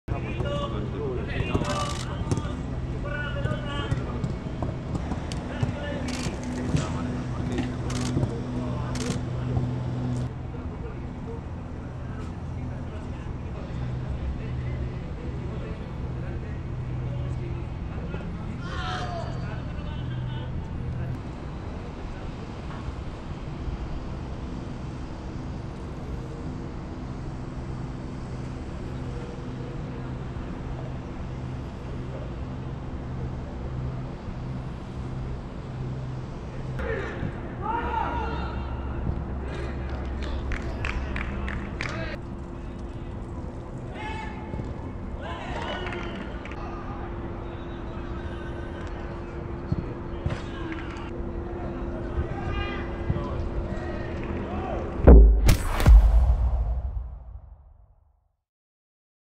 VUELTA A LOS ENTRENAMIENTOS PENSANDO EN EL PRÓXIMO PARTIDO
Un entrenamiento marcado por el calor, la exigencia y el ánimo de superación. Jason ya comenzó a trabajar en solitario en uno de los campos de la Ciutat Esportiva.

The team prepare for their match on Saturday against RC Celta de Vigo at Balaídos at 9:00 PM.